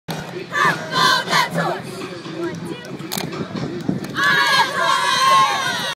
Ball High cheerleaders warm up for La Marque
Before the Clash of the Causeway season opener on Aug. 30.